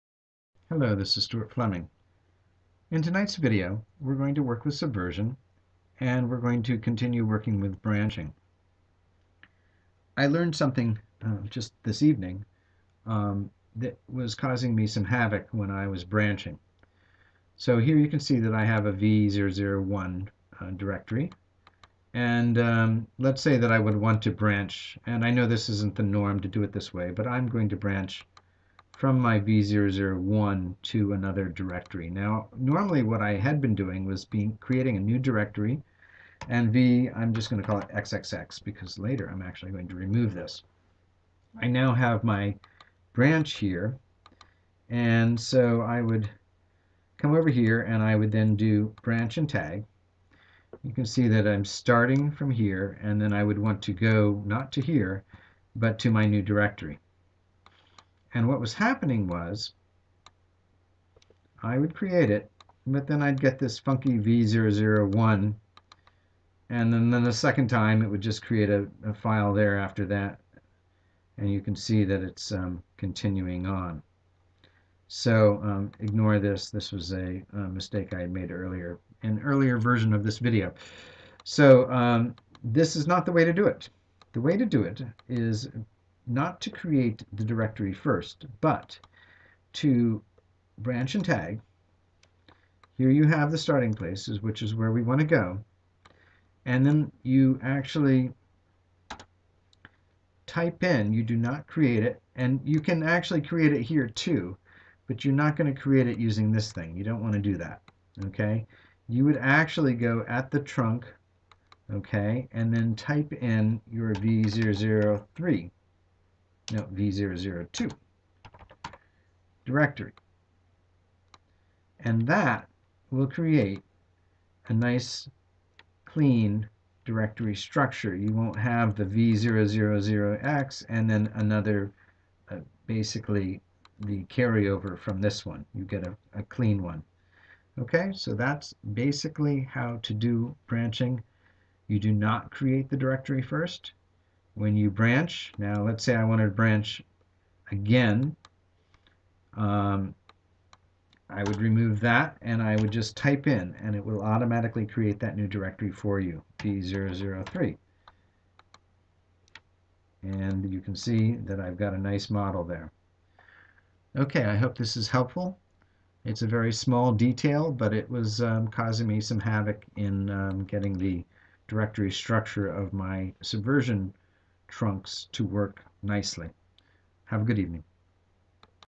052b Subversion Branching Working with Remote directories
In this video I show how if you first create a remote directory, the structure of the subversion tree is not "clean" since it inherits the main folder of the previous version.

Watch this exciting video for the juicy details on how to get the directory structure right!